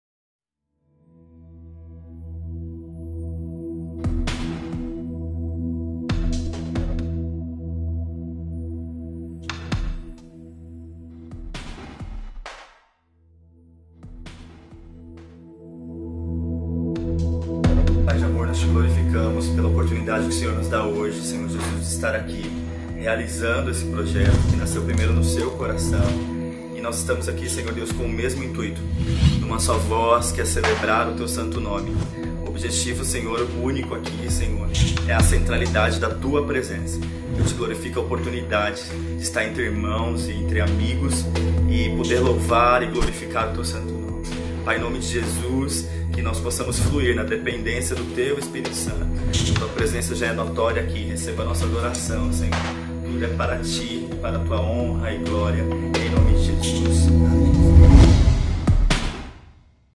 MAKING OF DA LIVE - SINGLE LIVRE
Galera confiram o primeiro making of do meu projeto,um pouco do que rolou antes de assistirem a LIVE do meu single,pra ficar com aquele gostinho de quero mais haha
É um sonho que antes de surgir em meu coração já estava no do Pai;e eu estou realizado com o resultado de TUDO e ainda mais por viver os Propósitos de Deus.